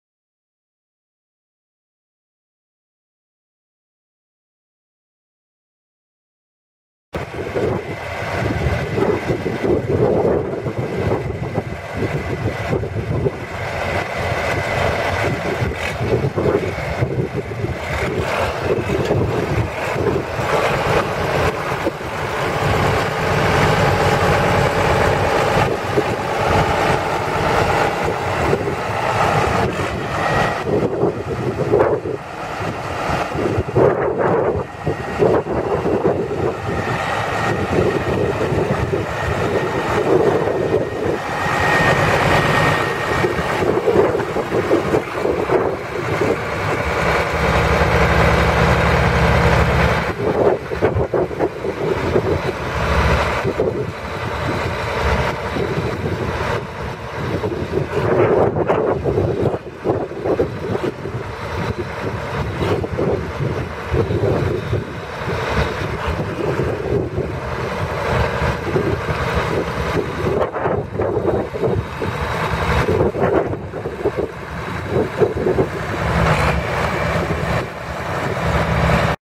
1974 Case 1270 tractor for sale | no-reserve Internet auction July 26, 2017
1974 Case 1270 tractor
3,803 hours on meter
Case six cylinder diesel engine
127 HP
Serial 2539824
Powershift transmission
12F-4R gears
Heat
Tilt steering wheel
AM
Suspension seat
Three point
Top link
1000 PTO
(2) rear hydraulic remote sets
11.00-16 front tires
20.8-38 dual rear tires
Non-operational PTO
Serial 8737906
It's located in Bunker Hill, KS, and we're selling it to the highest bidder regardless of price.